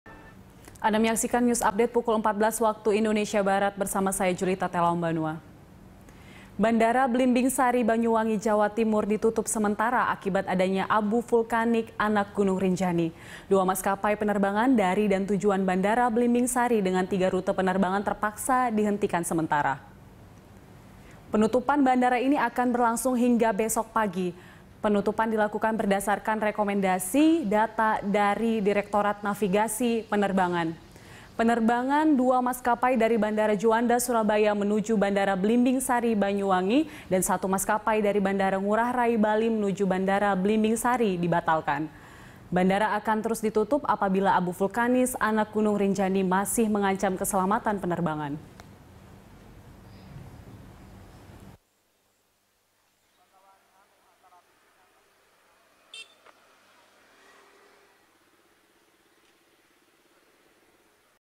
3 Rute Penerbangan Dihentikan Akibat Abu Vulkanik Rinjani
Bandara Blimbingsari Banyuwangi, Jawa Timur ditutup sementara akibat adanya abu vulkanik anak Gunung Rinjani. Dua maskapai penerbangan dari dan tujuan Bandara Blimbingsari dengan tiga rute penerbangan terpaksa dihentikan sementara.

Facebook.com/BeritaSatuTV
Youtube.com/BeritaSatu